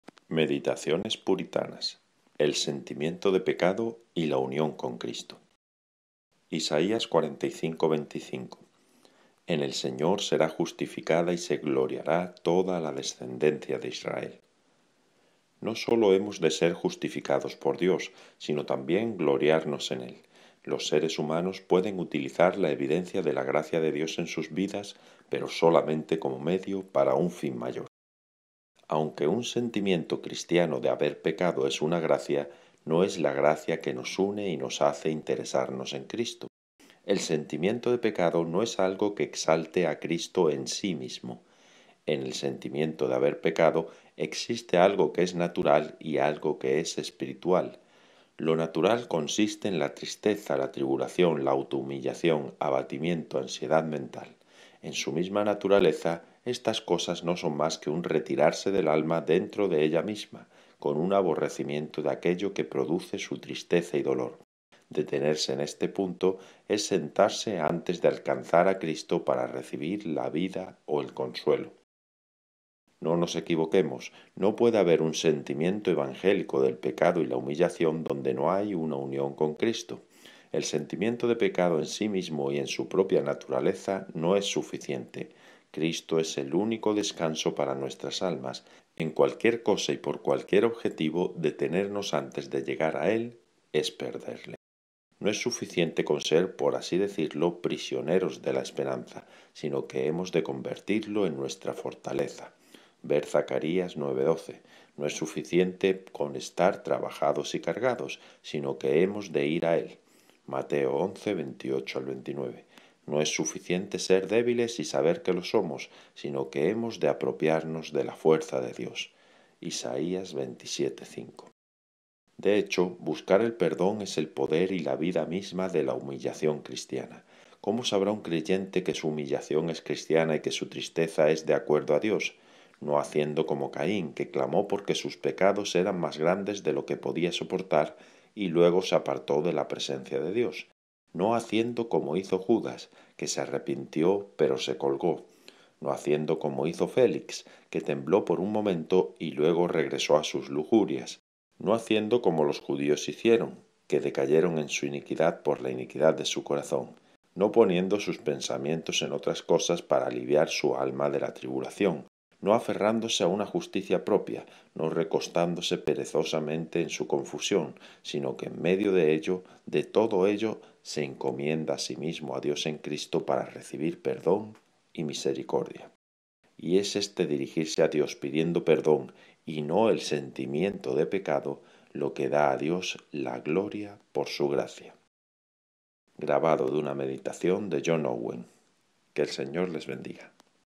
Meditaciones Puritanas - John Owen - El sentimiento de pecado y la unión con Cristo
Sentir dolor por haber pecado es ciertamente algo bueno. Una gracia dada por Dios. Pero no hemos de detenernos ahí, sino buscar la unión con Cristo para perdón. Una reflexión del puritano John Owen.